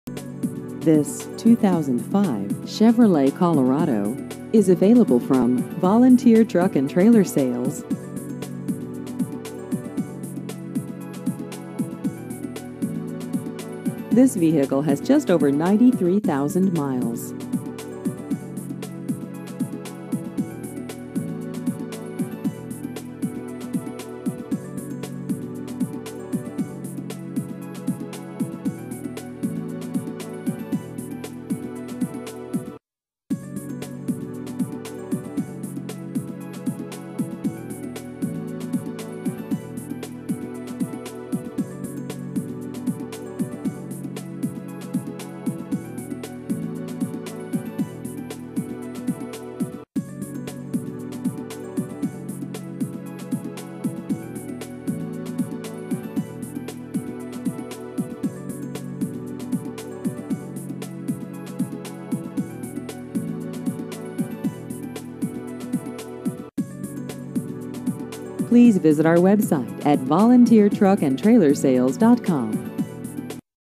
2005 Chevrolet Colorado Used Cars Maynardville and Tazewell
This 2005 Chevrolet Colorado is available from Volunteer Motors & Trailer Sales.